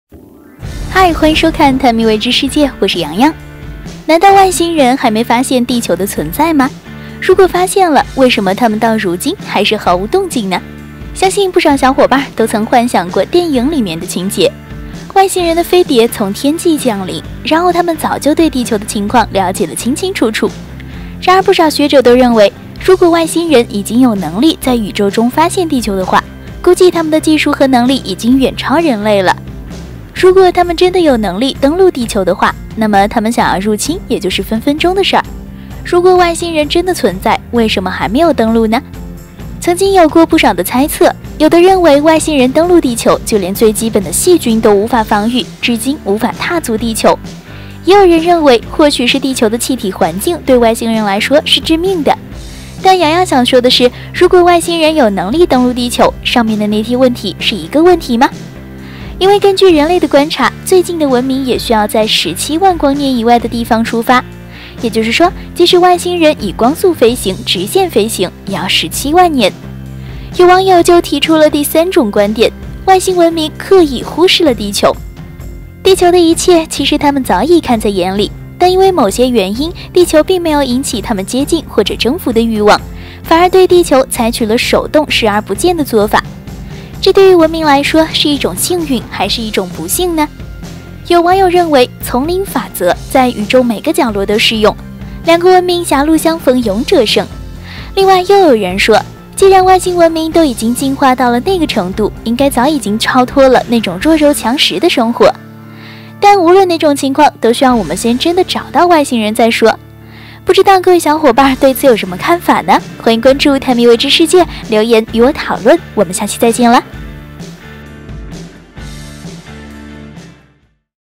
地球讓外星人懼怕？細菌對它們來說可能是致命因素！
假設宇宙中的確存在外星人，為什麼它們沒有登陸地球呢？是因為它們還沒有發現地球，還是發現了卻沒有能力登陸地球？最近有這樣的一類說法，外星人或許是故意忽略地球，為什麼這樣說呢？